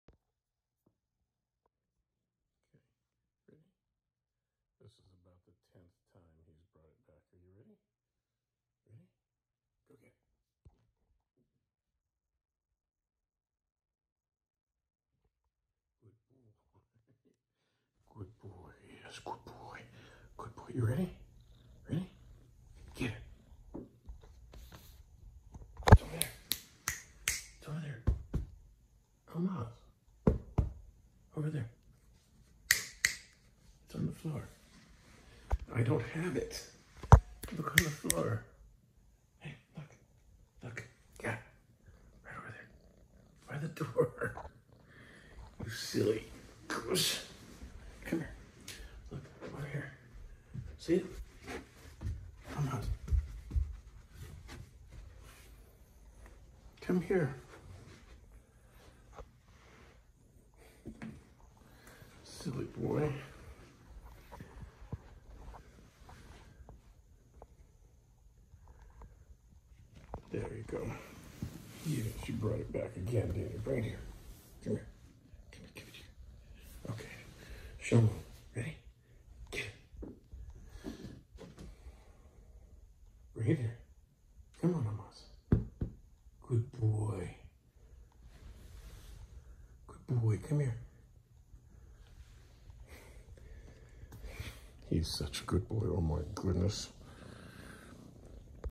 Bengal update, Our future Stud Boy Olnas
It's Bengal Time!

Pictured is Olnas our future stud boy playing his favorite game (Fetch). When I took this video we had already been playing for at least 10 minutes.

He is such a good boy and very affectionate.

Olnas will likely take Sascha's place when Sascha is ready to retire.  That is of course if he makes beautiful baby's like Sascha does.

Olnas was born in Alaska on 9-14-20.

Lyubov & Andy
Registered Bengals / Bengaltime Cattery
Kalama, Washington